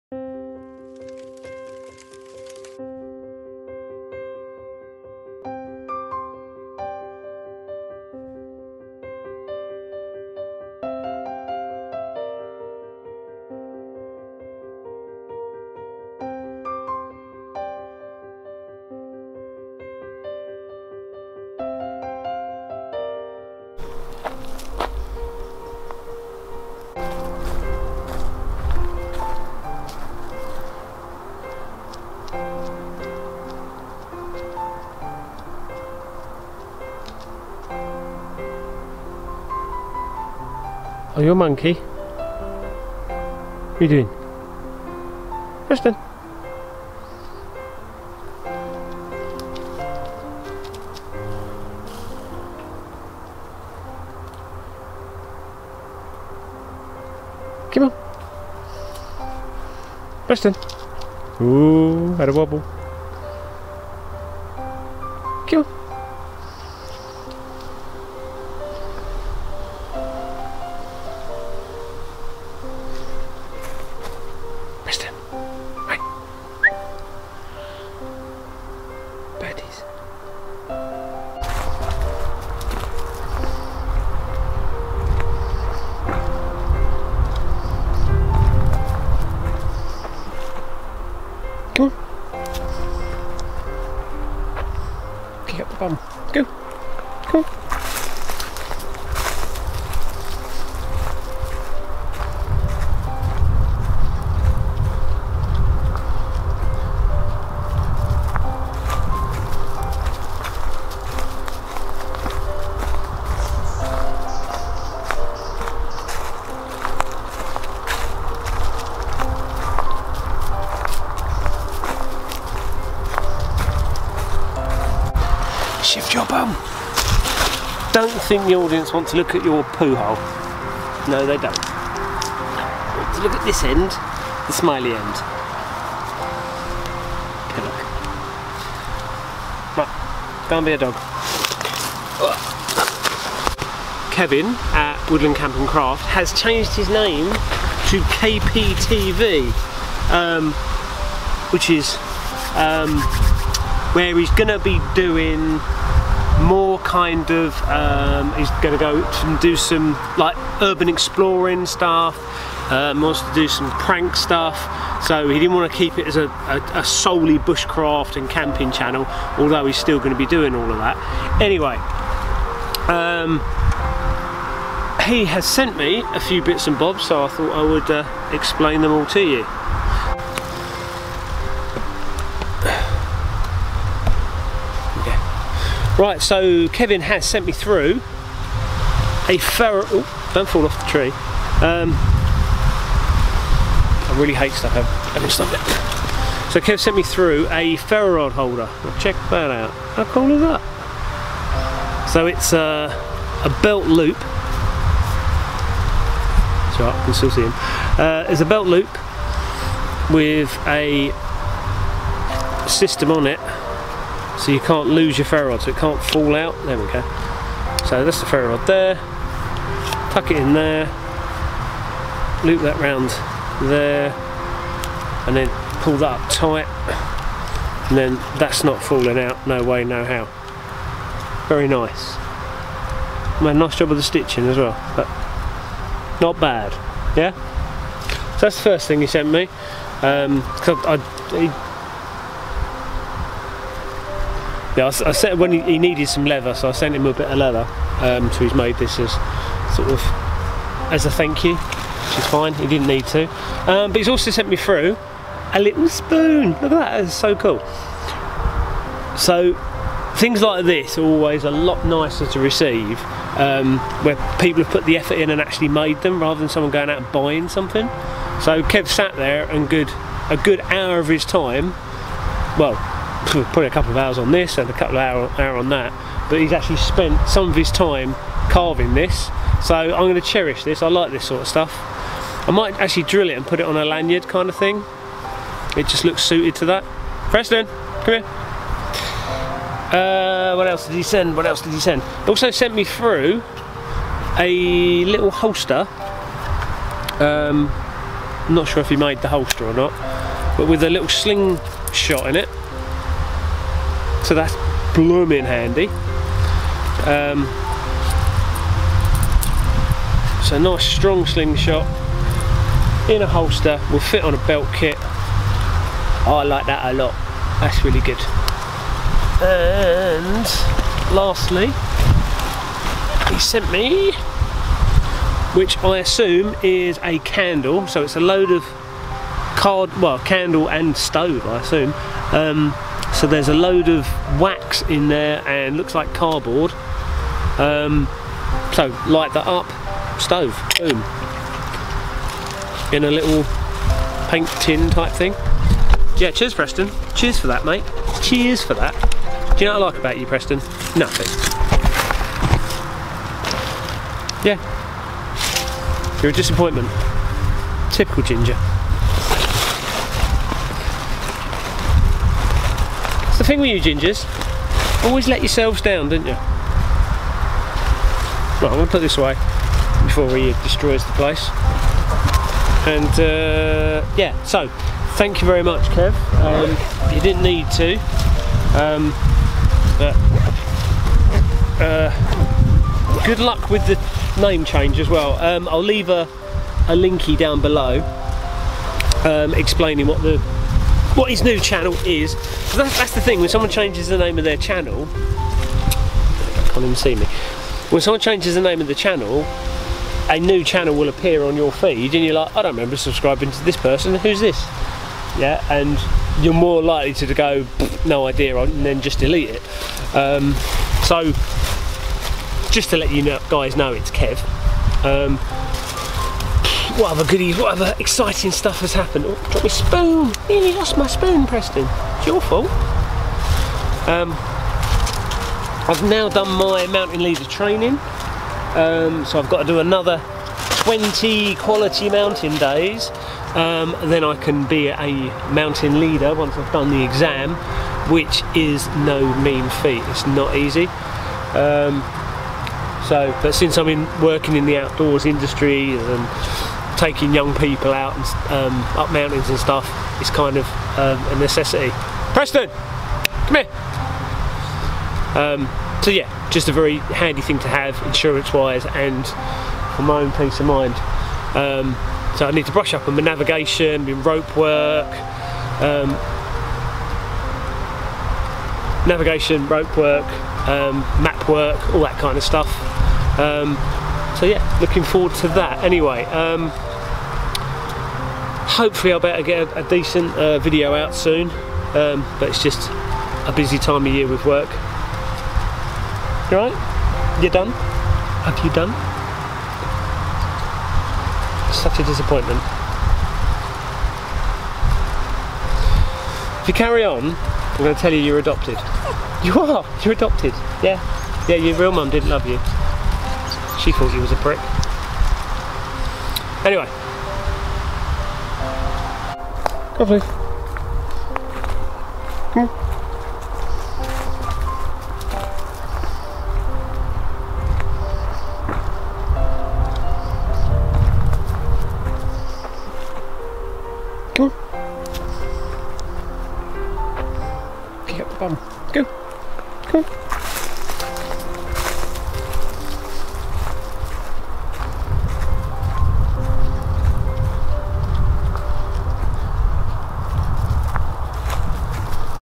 KPTV unboxing in the woods with cockapoo puppy
unboxing of some cool gifts from KPTV Squad, in the woods with my pesky Cockapoo puppy Preston...
Give him a sub guys! he was woodland camp and craft